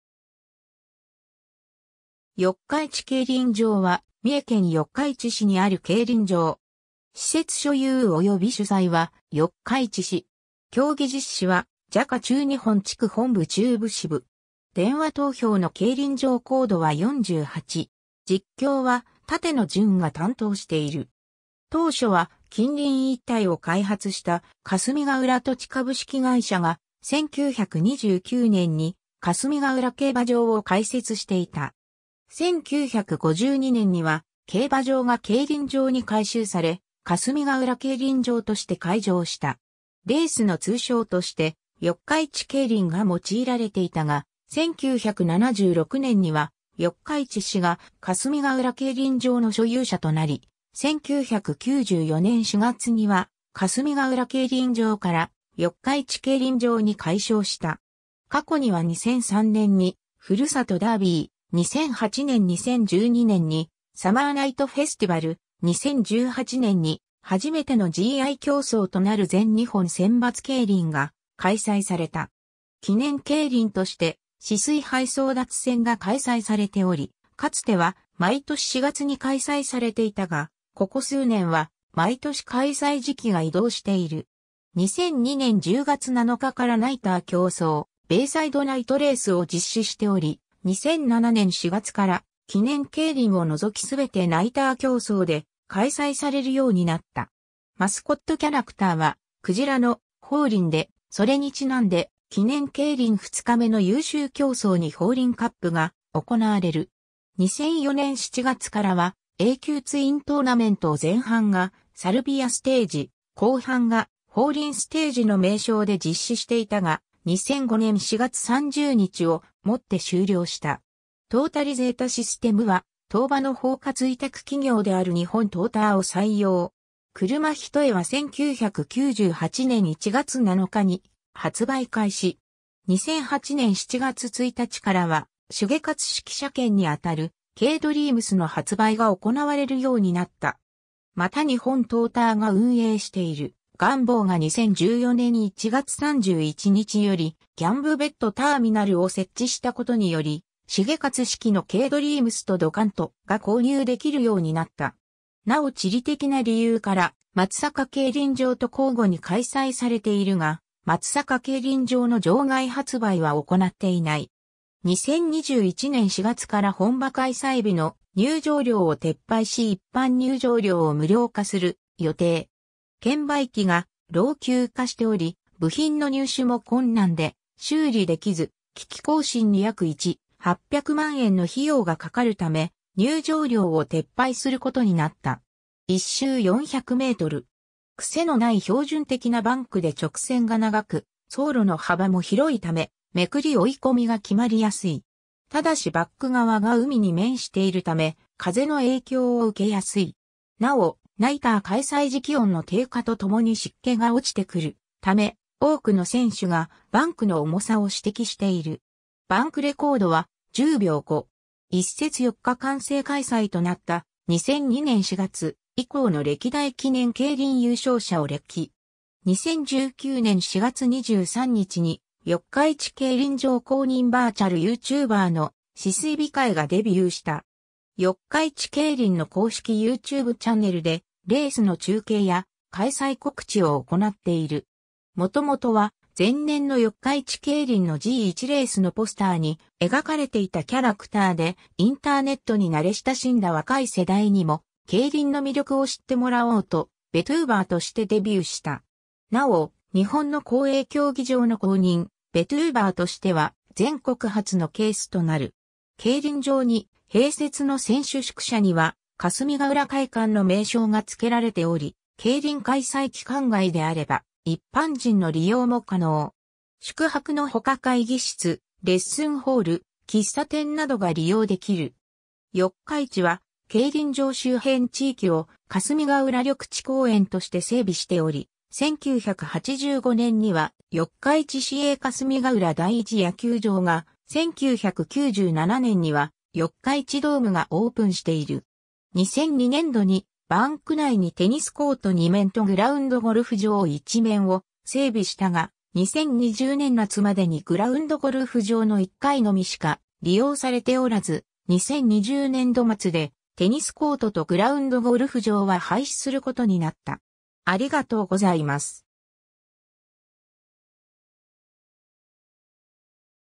四日市競輪場
四日市競輪場（よっかいちけいりんじょう）は三重県四日市市にある競輪場。
施設所有および主催は四日市市。
競技実施はJKA中日本地区本部中部支部。
電話投票の競輪場コードは48#。
実況は立野純が担当している。
 当初は近隣一帯を開発した霞ヶ浦土地株式会社が、1929年に霞ヶ浦競馬場を開設していた。
1952年には競馬場が競輪場に改修され、霞ヶ浦競輪場（かすみがうらけいりんじょう）として開場した。
レースの通称として「四日市競輪」が用いられていたが、1976年には四日市市が霞ヶ浦競輪場の所有者となり、1994年4月には霞ヶ浦競輪場から四日市競輪場（よっかいちけいりんじょう）に改称した。
 過去には2003年にふるさとダービー、2008年・2012年にサマーナイトフェスティバル、2018年に初めてのGI競走となる全日本選抜競輪が開催された。
記念競輪（GIII）として『泗水杯争奪戦』が開催されており、かつては毎年4月に開催されていたが、ここ数年は毎年開催時期が移動している。
2002年10月7日からナイター競走｢ベイサイドナイトレース」を実施しており、2007年4月から記念競輪を除き全てナイター競走で開催されるようになった（冬季のレースは寒さ対策として選手のパンスト着用も任意で可能）。
 マスコットキャラクターはクジラの「フォーリン」で、それにちなんで記念競輪2日目の優秀競走にフォーリンカップが行なわれる。
 2004年7月からはA級ツイントーナメントを前半が「サルビアステージ」・後半が「フォーリンステージ」の名称で実施していたが、2005年4月30日をもって終了した。
 トータリゼータシステムは当場の包括委託企業である日本トーターを採用。
車単（2車単･2車複）は1998年1月7日に発売開始。
2008年7月1日からは、重勝式車券にあたるKドリームスの発売が行われるようになった。
また日本トーターが運営している『Gamboo』が2014年1月31日より「GambooBETターミナル」を設置したことにより重勝式のKドリームスとDokanto!が購入できるようになった。
なお地理的な理由から松阪競輪場と交互に開催されているが、松阪競輪場の場外発売は行なっていない（川越場外発売所があるため。但し、払い戻しは可能）。
 2021年4月から本場開催日の入場料（1人50円）を撤廃し一般入場料を無料化する予定。
券売機が老朽化しており部品の入手も困難で修理できず、機器更新に約1,800万円の費用がかかるため入場料を撤廃することになった。
 1周400m。
クセのない標準的なバンクで直線が長く、走路の幅も広いため、捲り・追い込みが決まりやすい。
ただしバック側が海に面しているため、風の影響を受けやすい。
なお、ナイター開催時気温の低下と共に湿気が落ちてくるため、多くの選手がバンクの重さを指摘している。
 バンクレコードは、10秒5（テオ・ボス、2019年8月20日）。
 ※ 1節4日間制開催となった、2002年4月以降の歴代記念競輪優勝者を列記。
 2019年4月23日に、四日市競輪場公認バーチャルYouTuber（VTuber）の泗水美海（しすい みう）がデビューした。
四日市競輪の公式YouTubeチャンネルでレースの中継や開催告知を行っている。
 元々は前年の四日市競輪のG1レースのポスターに描かれていたキャラクターで、「インターネットに慣れ親しんだ若い世代にも競輪の魅力を知ってもらおう」とVTuberとしてデビューした。
なお、日本の公営競技場の公認VTuberとしては全国初のケースとなる。
 競輪場に併設の選手宿舎には「霞ヶ浦会館」の名称が付けられており、競輪開催期間外であれば一般人の利用も可能。
宿泊のほか会議室・レッスンホール・喫茶店などが利用できる（宿泊は原則として10名以上の団体が対象）。
 四日市は競輪場周辺地域を霞ヶ浦緑地公園として整備しており、1985年には四日市市営霞ヶ浦第一野球場が、1997年には四日市ドームがオープンしている。
 2002年度にバンク内にテニスコート二面とグラウンドゴルフ場一面を整備したが、2020年夏までにグラウンドゴルフ場の1回のみしか利用されておらず、2020年度末でテニスコートとグラウンドゴルフ場は廃止することになった。